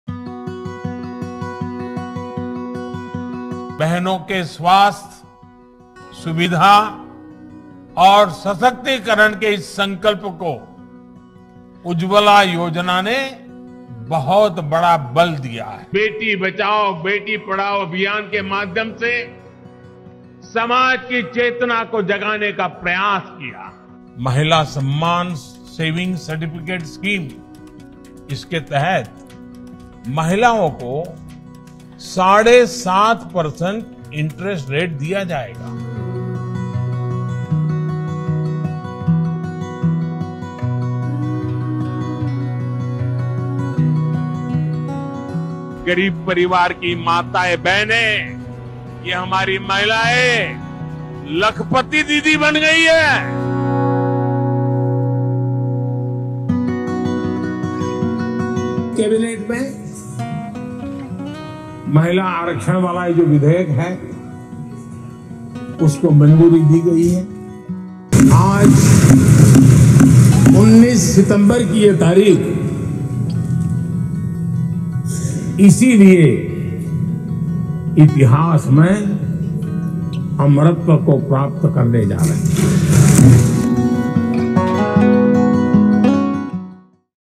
Union Cabinet has passed the historic Women's Reservation Bill
On Sept 19, 2023, the Union Cabinet passed the historic Women's Reservation Bill. The bill aims to provide 33% reservation to women in both the Parliament and state Legislatures.